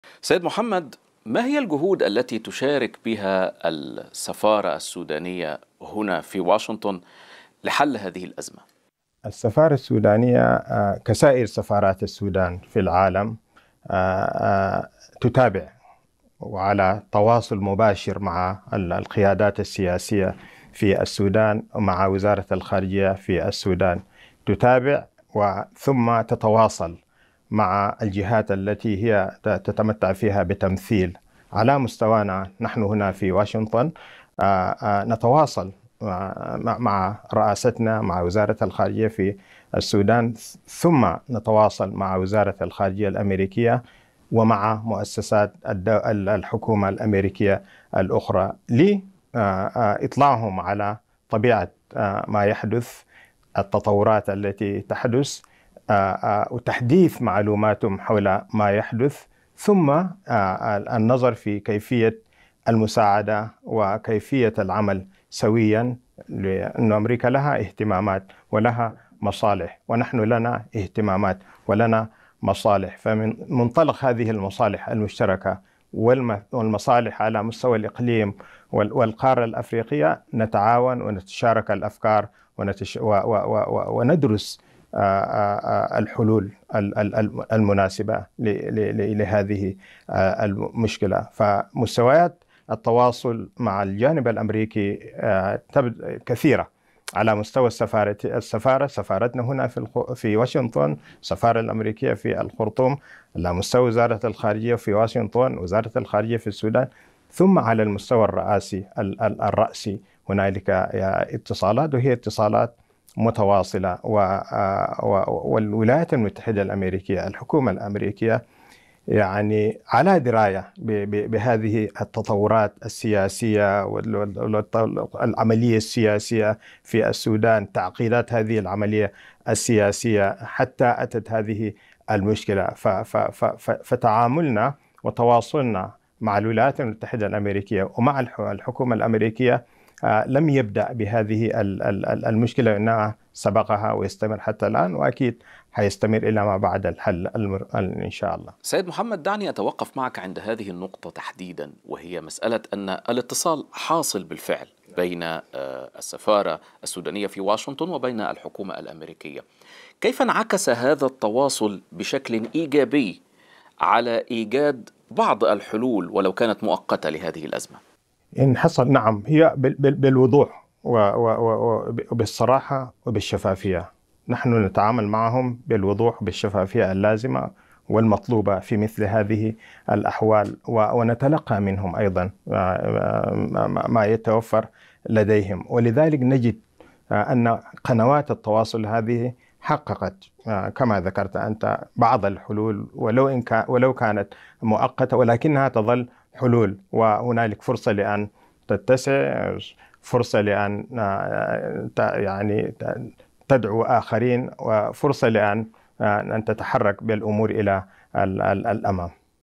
السفير السوداني بواشنطن: تقييمنا للأوضاع الأمنية يختلف عن أمريكا وسنعمل على إيجاد حل مستدام
محمد عبدالله إدريس السفير السوداني بواشنطن: تقييمنا للأوضاع الأمنية يختلف عن أمريكا وسنعمل على إيجاد حل مستدام

24/4/2023